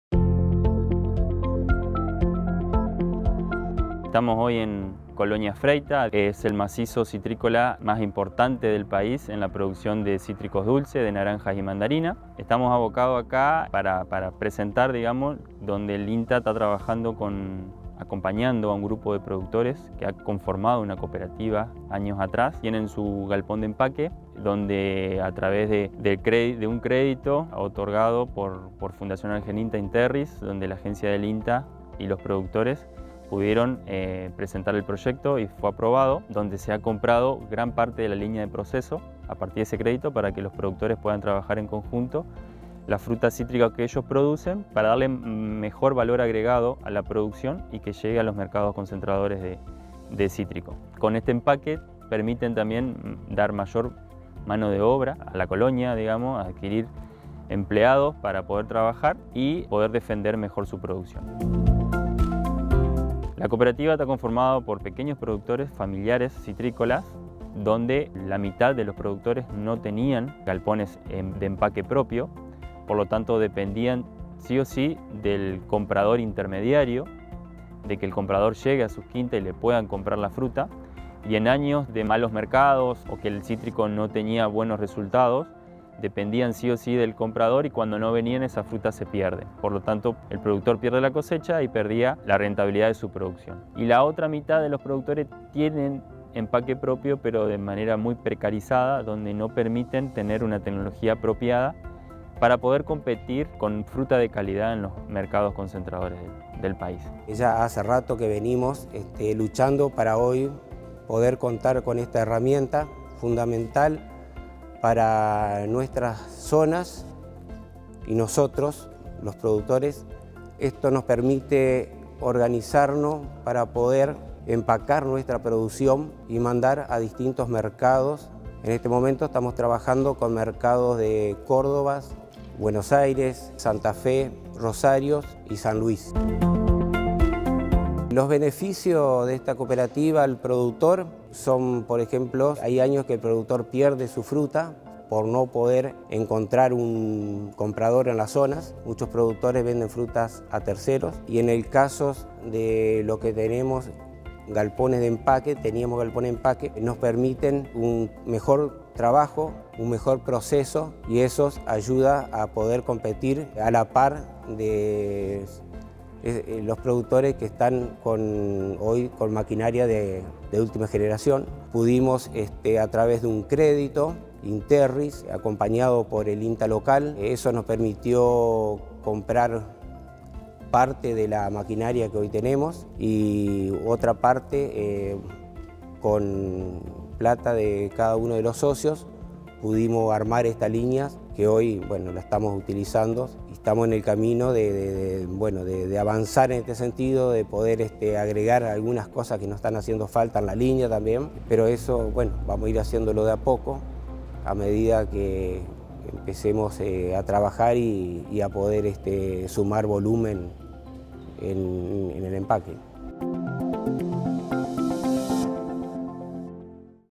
En esta cooperativa hay equipo
La Cooperativa de Productores Frutihortícolas Procítrus de Colonia Freitas, en Entre Ríos conformada con más de 10 asociados, con el acompañamiento del INTA Chajarí, obtuvo un crédito para la compra de gran parte de las máquinas procesadoras de cítricos dulces. Esto le permitirá dar valor a las frutas y venderlas directamente a los mercados concentradores del país.